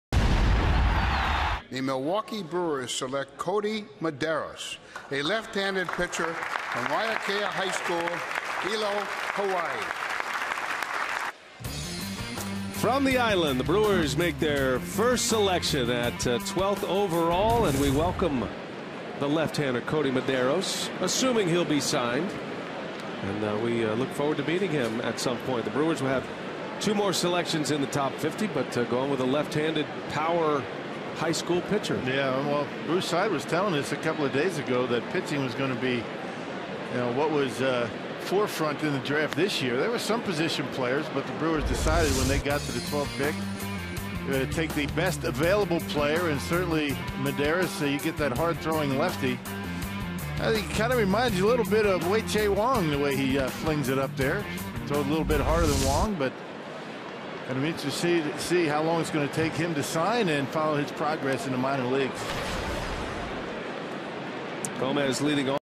MIL@MIN: Medeiros introduced as Brewers' draft pick
6/5/14: Left-handed pitcher Kodi Medeiros is shown being announced as the Brewers' first-round pick at the 2014 MLB Draft